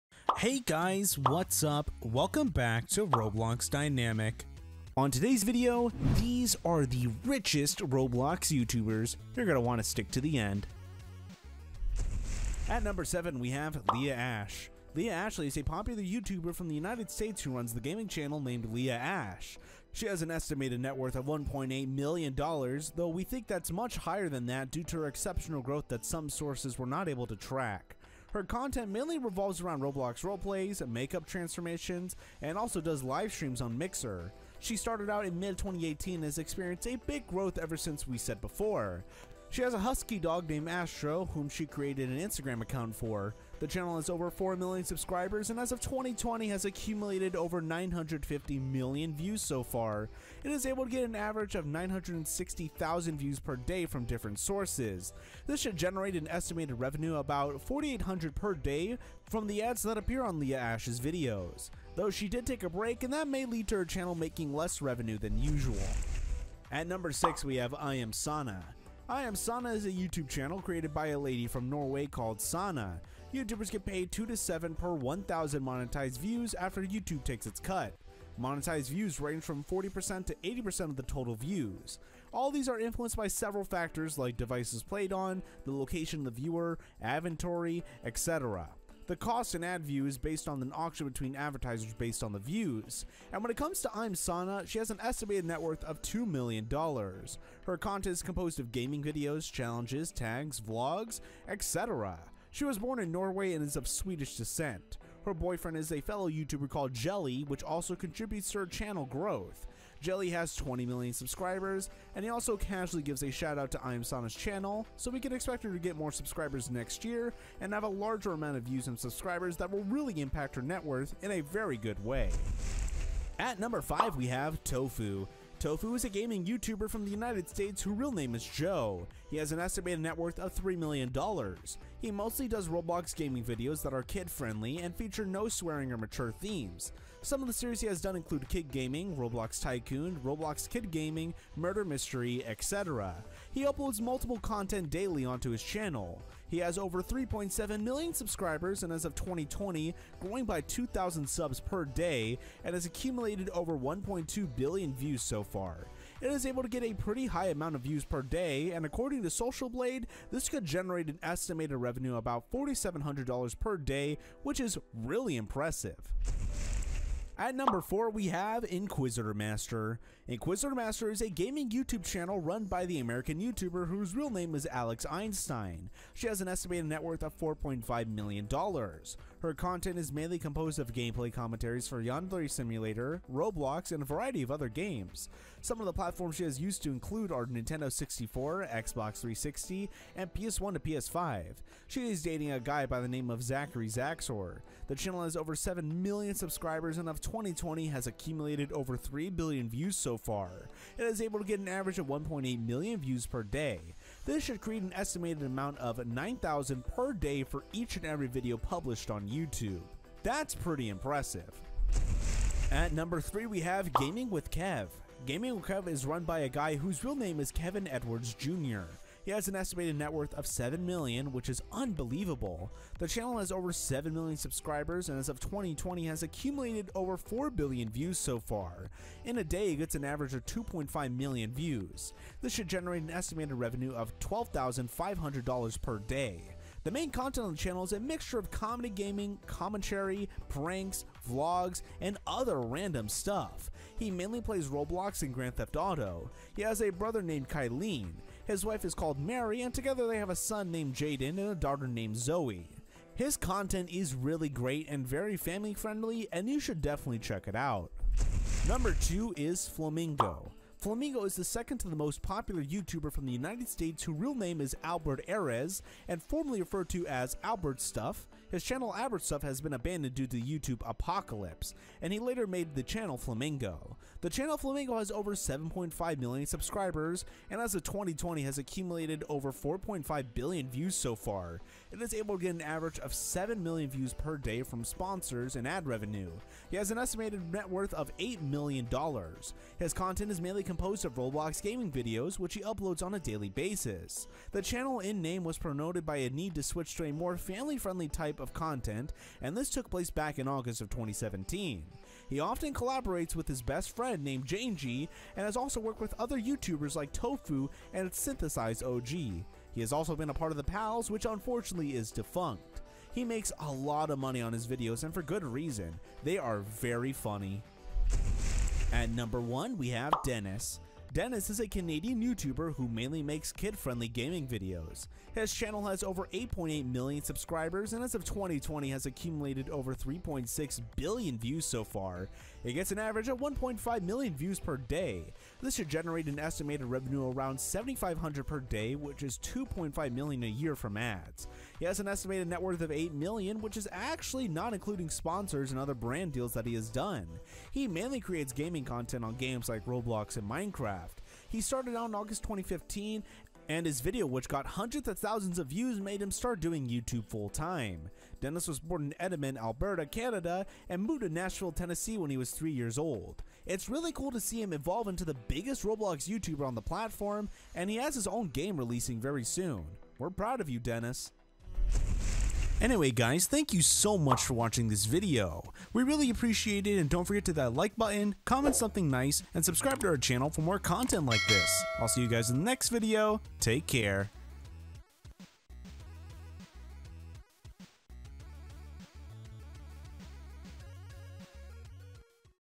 These Are The RICHEST Roblox YouTubers of ALL TIME!!
Today's video is These Are The RICHEST Roblox YouTubers of ALL TIME!! Hope you guys enjoy!
We Make videos about Roblox !
▶ Other Videos I LOVE To Watch